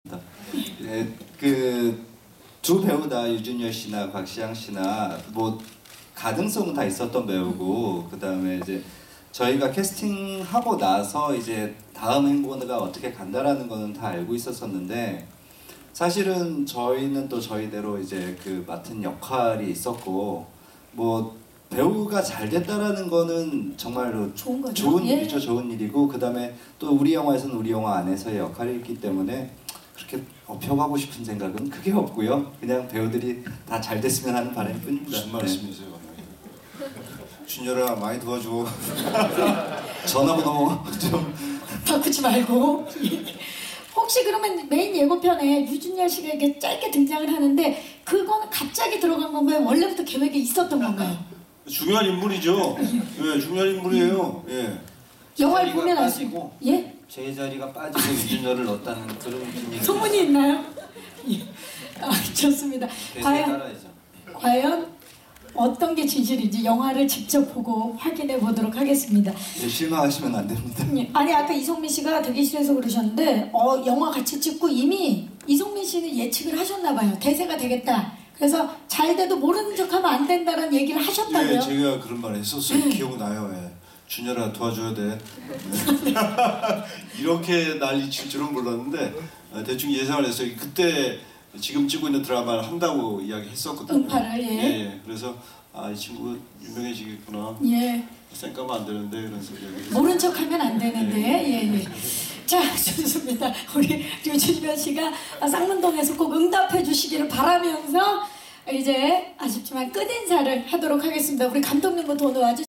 류준열 깜짝 출연에 대해 [영화] 로봇,소리 제작보고회
2015년 12월 14일(월) 오전, 서울 롯데시네마 건대입구에서 열린 영화 '로봇,소리' 제작보고회 현장. 감독: 이호재 주연: 이성민, 이희준, 이하늬, 김원해, 채수빈, 심은경(목소리) [제공/배급:  롯데엔터테인먼트 제작: 영화사 좋은날, 디씨지플러스 홍보: 영화인] [박재환@KBS미디어]